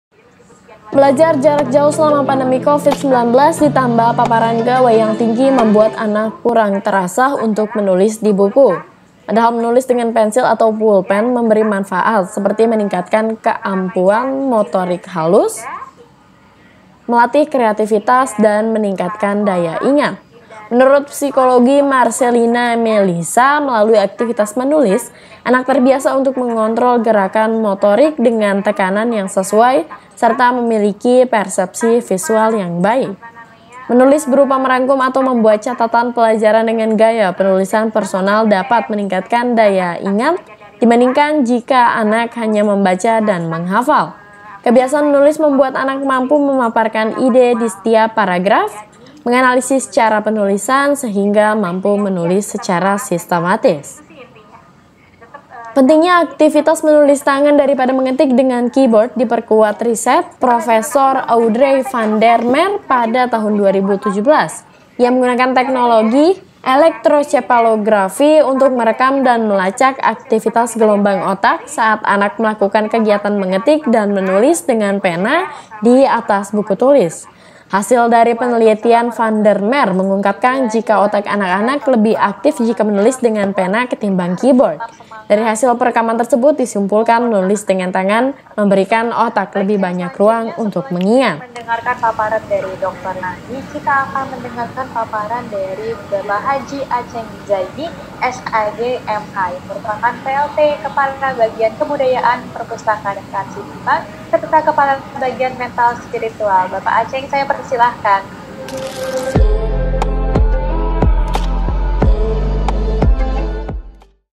Menulis di Buku Dapat Melatih Daya Ingat
Belajar jarak jauh selama pandemi Covi-19, ditambah paparan gawai yang tinggi membuat anak kurang terasah untuk menulis di buku. Padahal menulis dengan pensil/pupen memberi manfaat seperti, menikatkan keampuan motorik halus, melatih kreativitas, dan meningkatkan daya ingat. Menurut psikolog Marcelina Melisa, M.Psi, melalui aktivitas menulis, anak terbiasa untuk mengontrol gerakan motorik dengan tekanan yang sesuai, serta memiliki persepsi visual yang baik. “Anak juga dapat mengembangkan imajinasi dan kreativitas melalui ide tulisan,” ujar dia dalam webinar bertajuk “Pentingnya Menjaga Minat Menulis Anak,” yang diadakan oleh SiDu Kamis (3/12).

Reporter: Haryo Brono
Editor: Rizky Hari